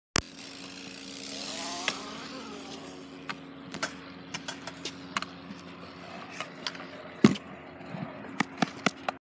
Jumping a gap at the skate park
It was big for me